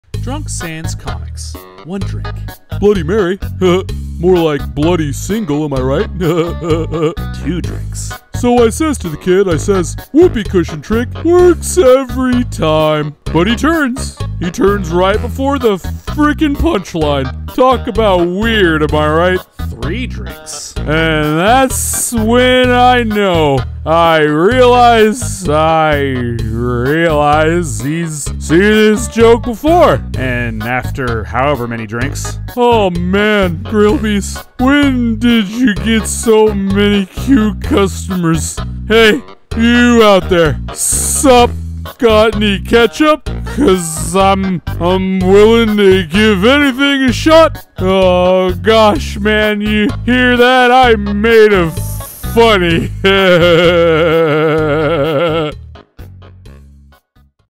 Sans on Ketchup! Undertale Comic Dub!
Sans loves his ketchup! But how does he act when he drinks?
Not sure who the artist for this one is. If you happen to know, please tell me so i can give them credit! Heres where i found the comic: